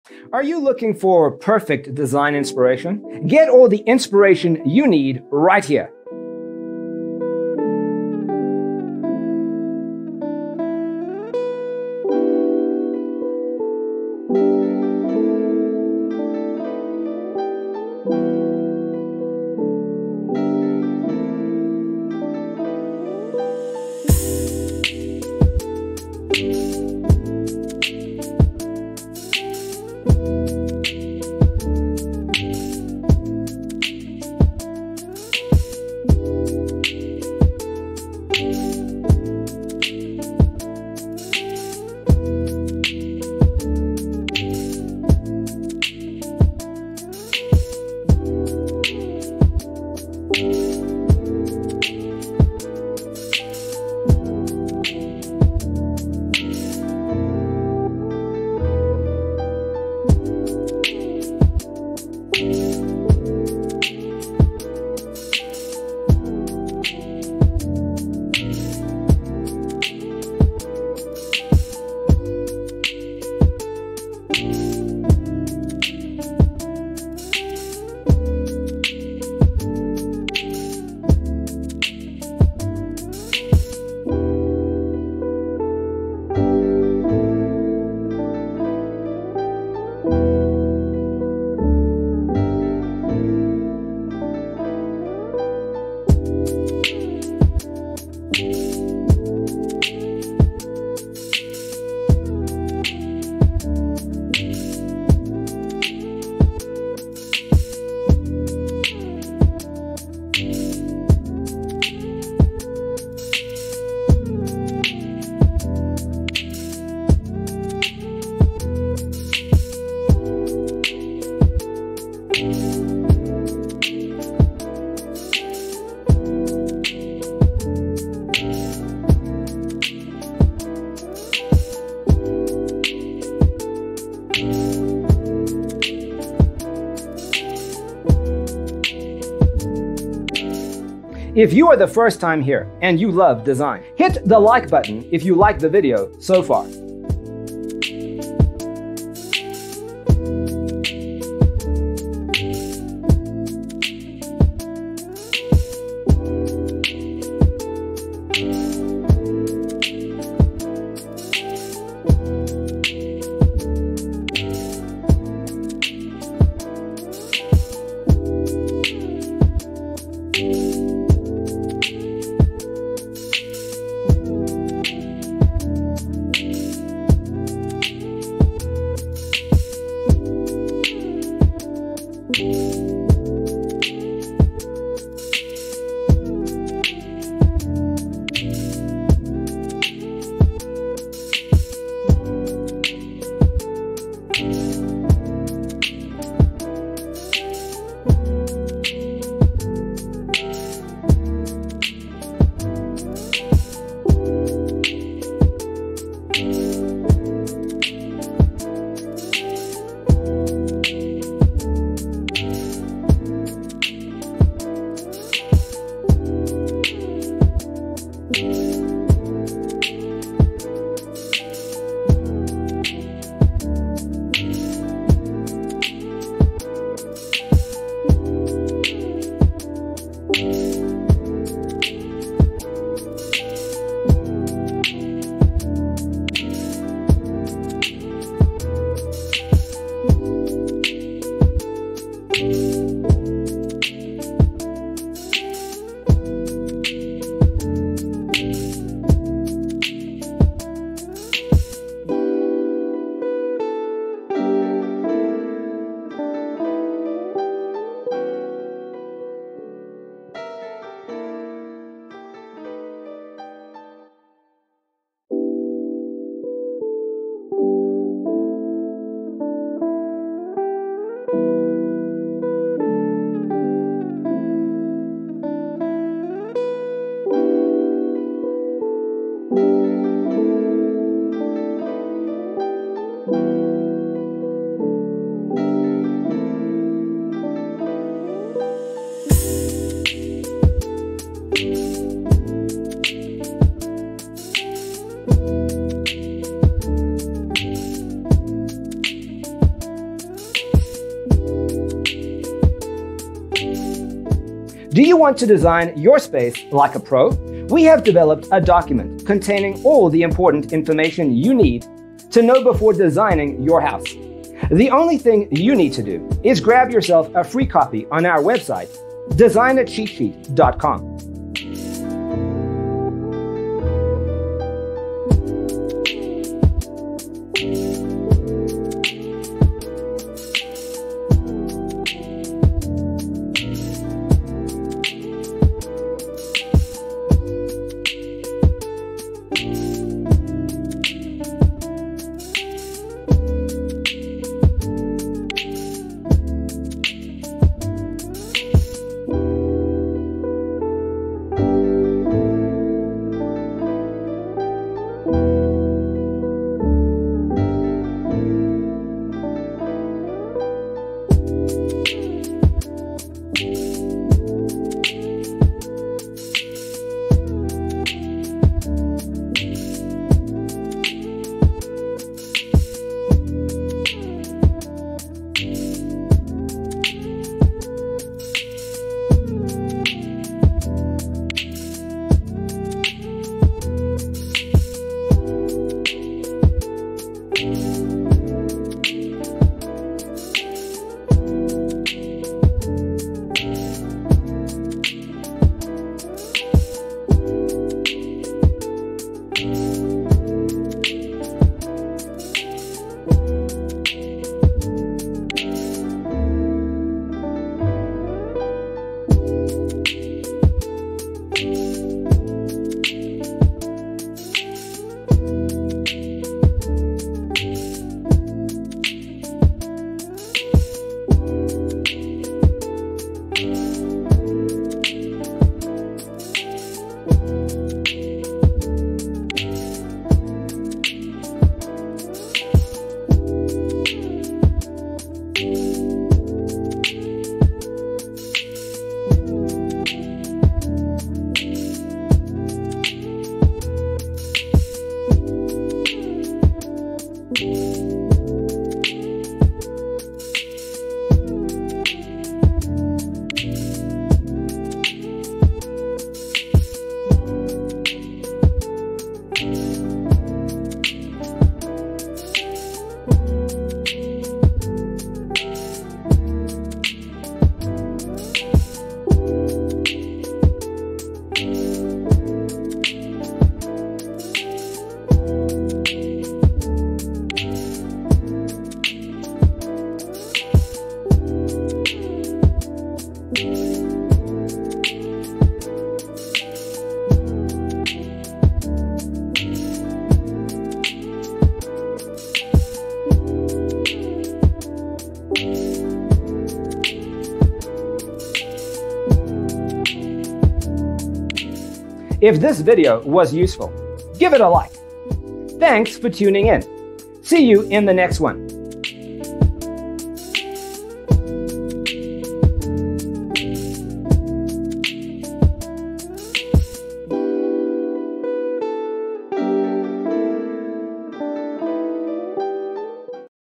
75 Mediterranean Home Gym Design Ideas You'll Love ➤
Browse Mediterranean home gym pictures. Discover new Tuscan home gym ideas, designs, decor and layouts to enhance your exercise routine.

0:00:00 - Intro
0:00:12 - Mediterranean Home Gym Design Ideas You'll Love: Inspirations 1 to 25
0:03:03 - Mediterranean Home Gym Design Ideas You'll Love: Inspirations 26 to 50
0:06:07 - Mediterranean Home Gym Design Ideas You'll Love: Inspirations 51 to 75

👋 Well Hello!